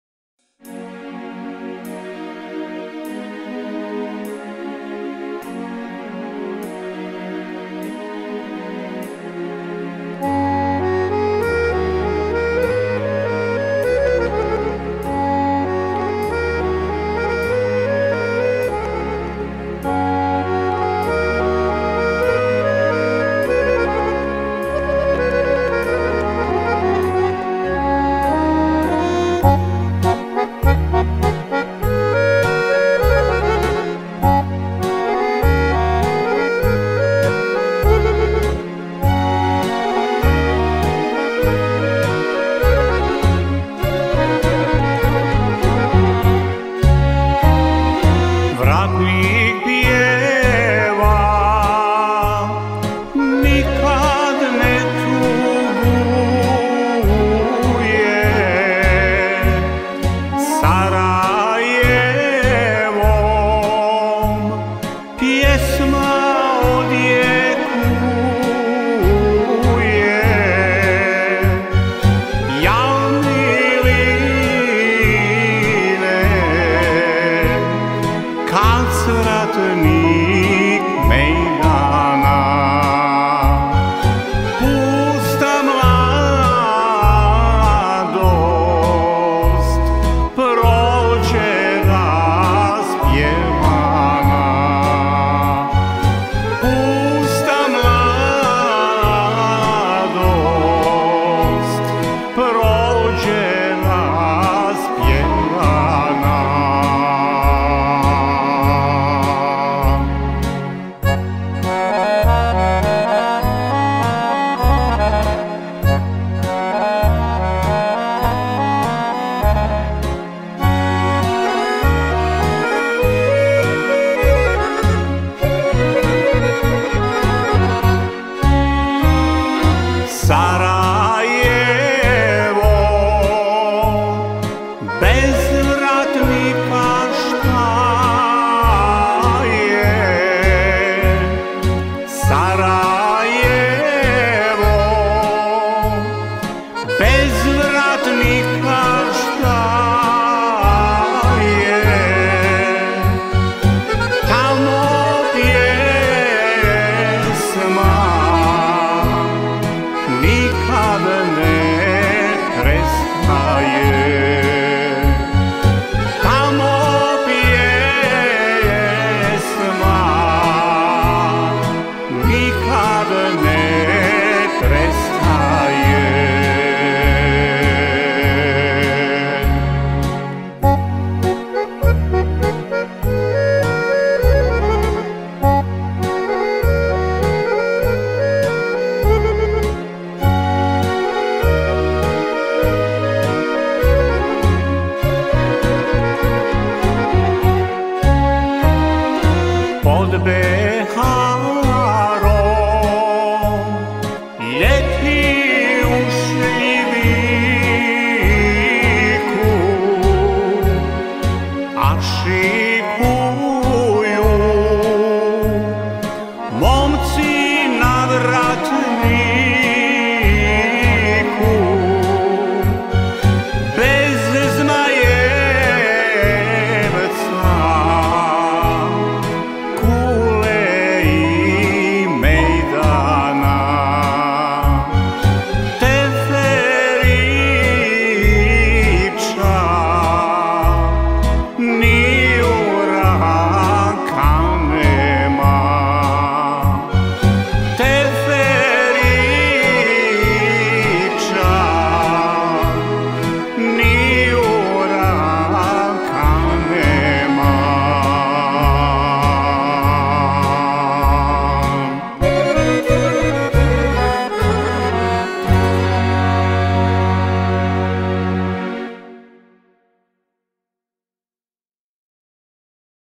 VRATNIK PJEVA
MUHAMED KOČAN SEVDALINKE KOJE VOLIMO.
 ARANŽMAN I MUZIČKA PRATNJA:ZLATKO ZLAJA SJERIĆ, "SJERIĆ RECORDS"